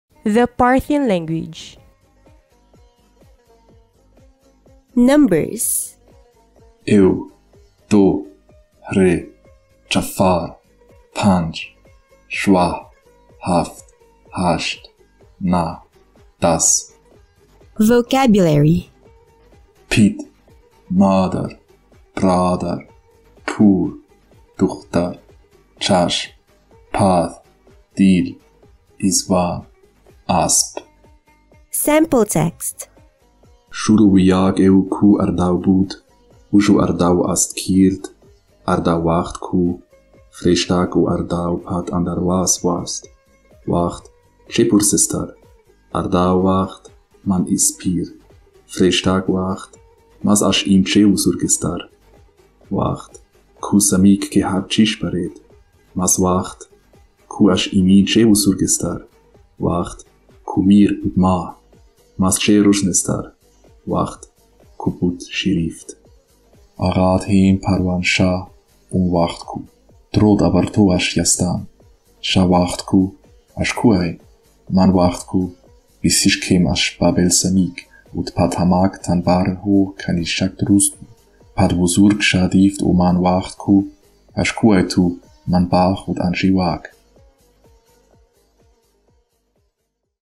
The Sound of the Parthian Language (Numbers, Words & Sample Text)
Special Thanks to Anti dweller :D

Parthian / Arsacid Pahlavi (Pahlawānīg)
Native to: Parthian Empire (incl. Arsacid dynasty of Armenia, Arsacid dynasty of Iberia and Arsacid Dynasty of Caucasian Albania)
Region: Parthia, ancient Iran
Era: State language 248 BC – 224 AD. Marginalized by Middle Persian from the 3rd century, though longer existent in the Caucasus due to several eponymous branches
Language family: Indo-European (Parthian
Arsacid Pahlavi
Pahlawānīg
Language family
Indo-European (Indo-Iranian)

The Parthian language, also known as Arsacid Pahlavi and Pahlawānīg, is a now-extinct ancient Northwestern Iranian language spoken in Parthia, a region situated in present-day northeastern Iran and Turkmenistan. Parthian was the language of state of the Arsacid Parthian Empire (248 BC – 224 AD), as well as of its eponymous branches of the Arsacid dynasty of Armenia, Arsacid dynasty of Iberia, and the Arsacid dynasty of Caucasian Albania.

This language had a significant impact on Armenian, a large part of whose vocabulary was formed primarily from borrowings from Parthian; its derivational morphology and syntax was also affected by language contact, but to a lesser extent. Many ancient Parthian words were preserved, and now only survive in Armenian.

Parthian was a Western Middle Iranian language. Language contact made it share some features of the Eastern Iranian language group, the influence of which is attested primarily in loanwords. Some traces of Eastern influence survive in Parthian loanwords in Armenian. Parthian loanwords appear in everyday Armenian vocabulary; nouns, adjectives, adverbs, denominative verbs, and administrative and religious lexicons.

Taxonomically, Parthian, an Indo-European language, belongs to the Northwestern Iranian language group while Middle Persian belongs to the Southwestern Iranian language group.

If you are interested to see your native language/dialect to be featured here.